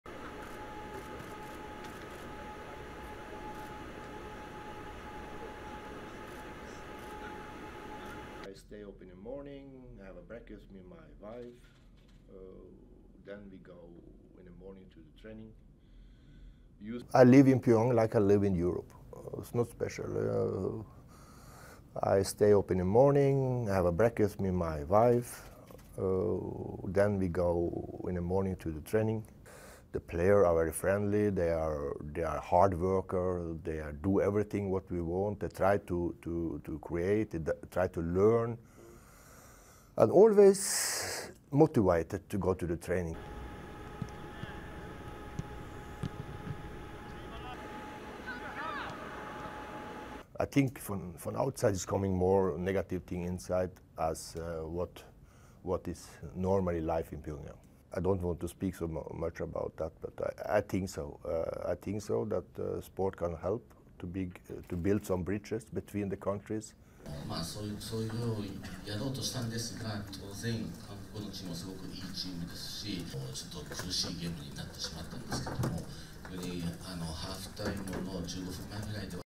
Jorn Andersen, sélectionneur "content" d'être en Corée du Nord
Il est le sélectionneur de l'équipe nationale d'un des pays les plus secrets et menaçants au monde pour beaucoup: pourtant, Jorn Andersen dit à l'AFP qu'il est "très content" d'avoir choisi la Corée du Nord et apprécie notamment la motivation de ses joueurs. IMAGES ET SONORES 01:19